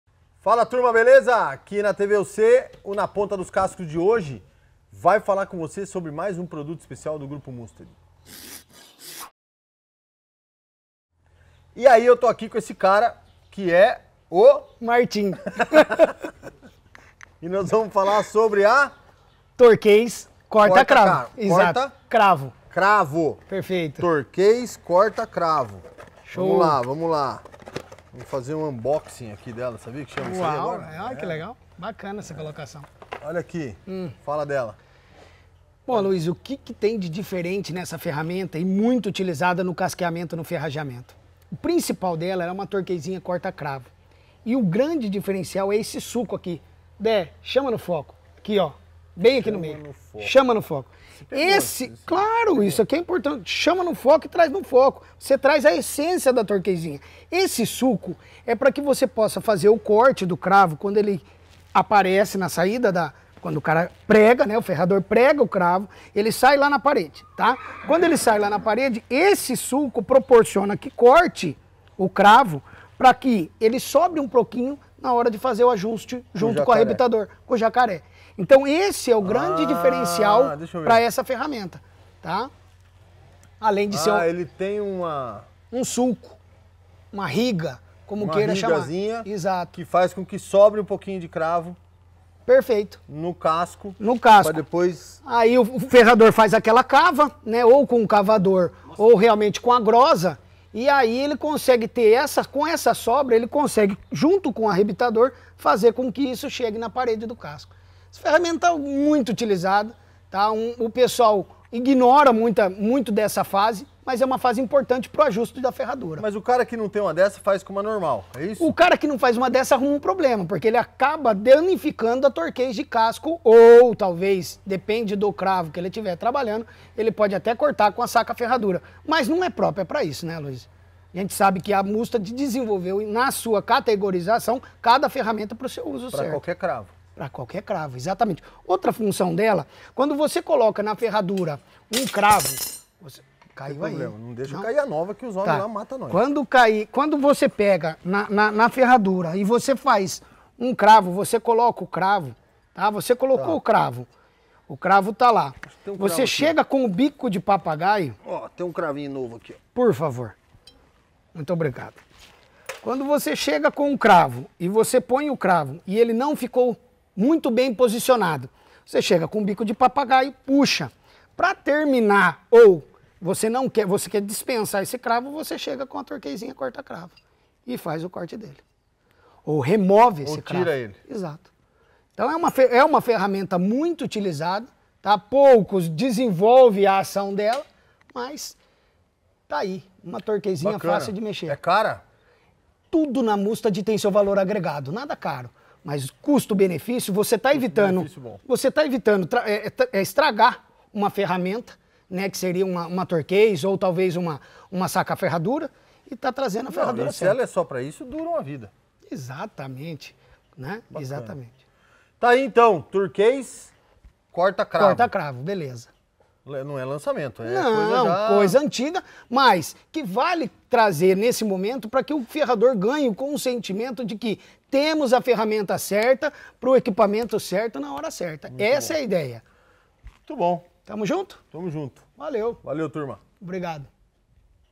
TV UC - Torquês corta-cravo
Uma ferramenta de uso específico e que é fundamental no bom acabamento dos cascos com ferraduras!

Conheça o nosso Curso de Casqueamento & Ferrageamento Online com Edison Pagoto

Informações sobre cursos:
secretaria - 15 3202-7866